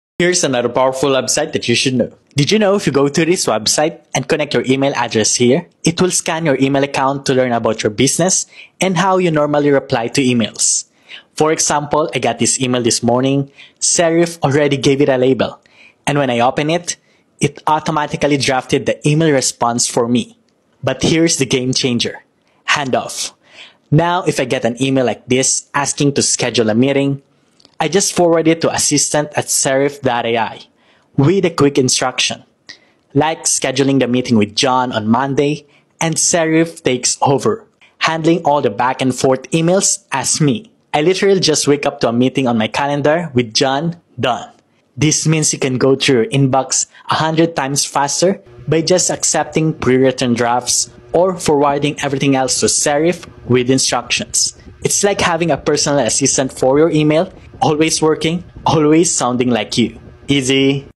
This AI Clears Your Inbox 100x Faster! (Your Personal Email Assistant!)
Drowning in emails?

Discover Serif.ai, the game-changing website that learns your reply style, drafts responses, and even handles complex follow-ups like scheduling meetings for you – literally clearing your inbox 100 times faster!

In this video, I'll show you:

1. How it scans your email to learn your unique communication style.
2. Automatic email drafting and labeling for incoming messages.
3. The revolutionary 'Handoff' feature: just forward an email with a quick instruction (like 'schedule meeting with John on Monday'), and Serif.ai takes over the entire back-and-forth, updating your calendar automatically!

It's like having a personal assistant dedicated to your email, always working and always sounding like you. Stop wasting hours on your inbox and start maximizing your productivity!